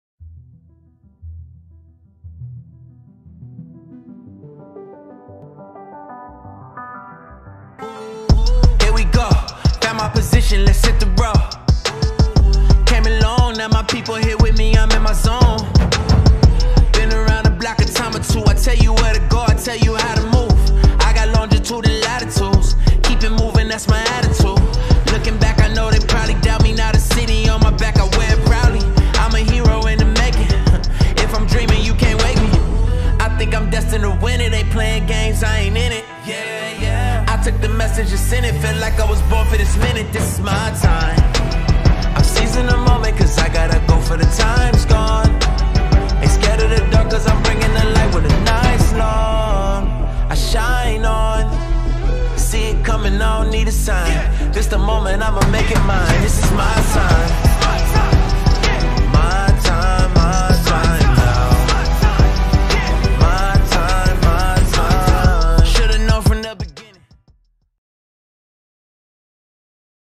SPEEDPAINT: Spider-Man - Miles Morales
hi, welcome to a video where i draw really quickly (but not really)
welp enjoy the video! owo
Spider-man: MARVEL
Audio/Music: Lecrae
Art, Video, and Editing: @JustTheTMG (me lol)
also merry christmas lol